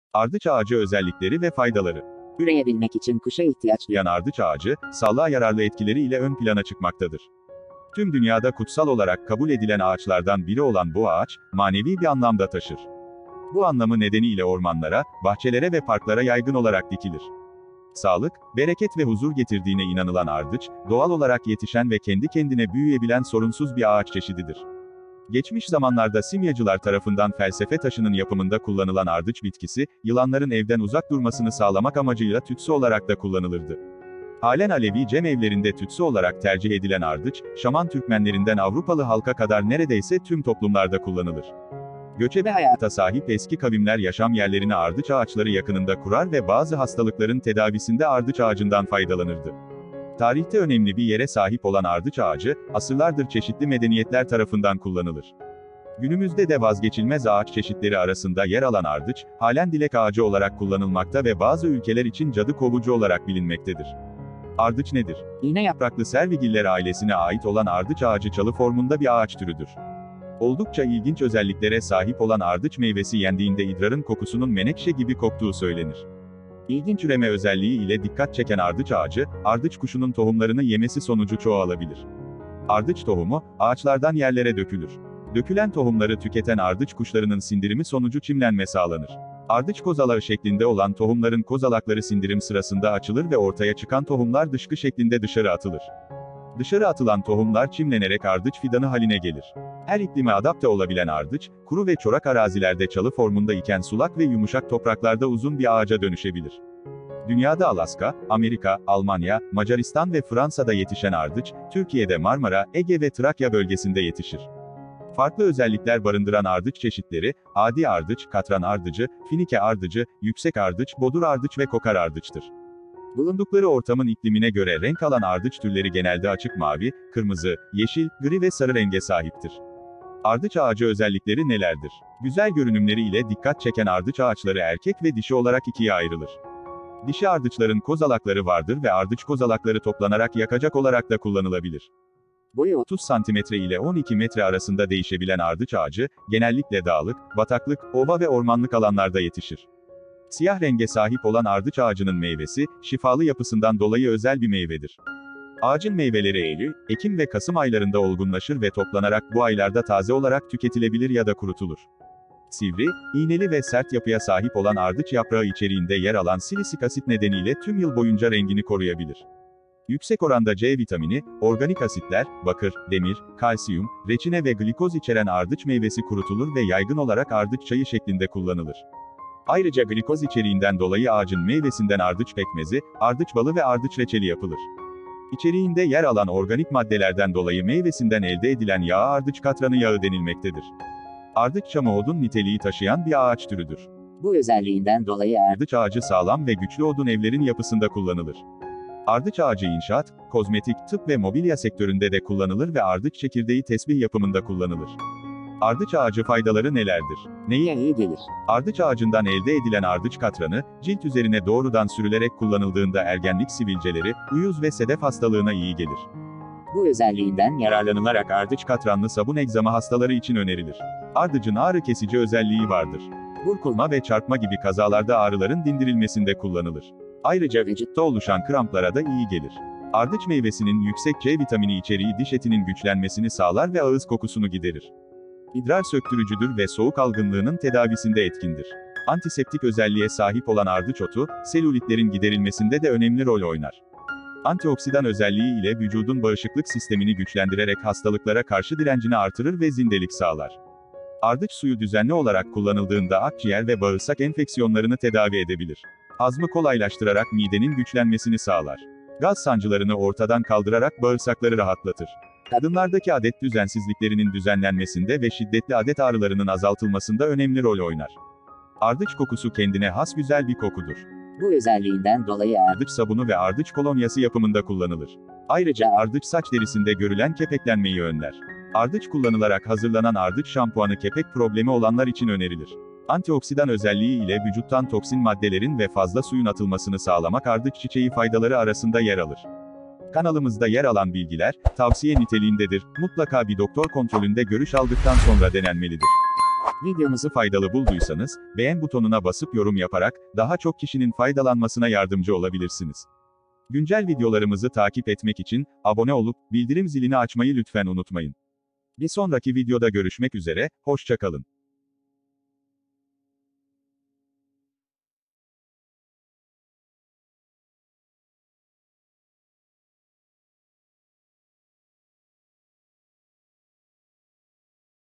Ardıç Ağacı Özellikleri ve Faydaları - Pratik Bilgi Bankası
Ardıç Ağacının Şifalı Dünyasını Keşfediyoruz
Üreyebilmek için kuşa ihtiyaç duyan ardıç ağacı, sağlığa yararlı etkileri ile ön plana çıkmaktadır. Tüm dünyada kutsal olarak kabul edilen ağaçlardan biri olan bu ağaç, manevi bir anlam da taşır. Bu anlamı nedeni ile ormanlara, bahçelere ve parklara yaygın olarak dikilir. Sağlık, bereket ve huzur getirdiğine inanılan ardıç, doğal olarak yetişen ve kendi kendine büyüyebilen sorunsuz bir ağaç çeşididir.

Geçmiş zamanlarda simyacılar tarafından felsefe taşının yapımında kullanılan ardıç bitkisi, yılanların evden uzak durmasını sağlamak amacıyla tütsü olarak da kullanılırdı. Halen Alevi Cem Evleri’nde tütsü olarak tercih edilen ardıç, Şaman Türkmenlerinden Avrupalı halka kadar neredeyse tüm toplumlarda kullanılır.
İçindekiler
Ardıç Nedir?
Ardıç Ağacı Özellikleri Nelerdir?
Ardıç Ağacı Faydaları Nelerdir? Neye İyi Gelir?
Ardıç Yağının Faydaları Nelerdir?
Ardıç Sirkesi Faydaları Nelerdir?
Ardıç Yağı Nasıl Kullanılır?